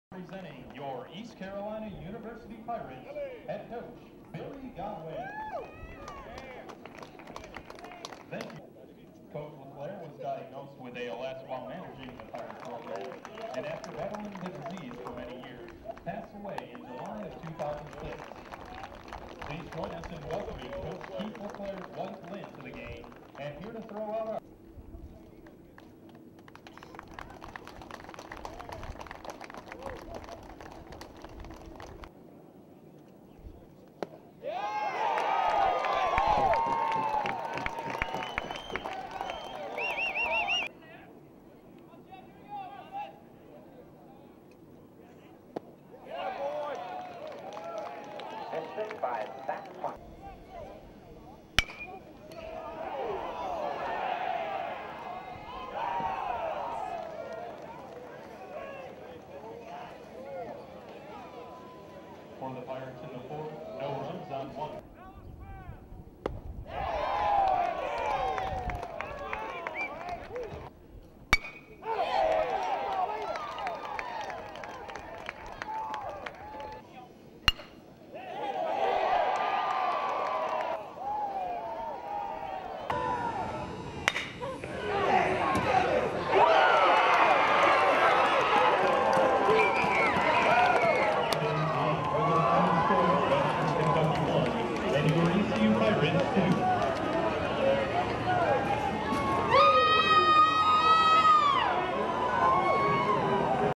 Keith LeClair Classic ECU vs WKU
ECU tops Western Kentucky in day one of the Keith LeClair Classic, 2-1.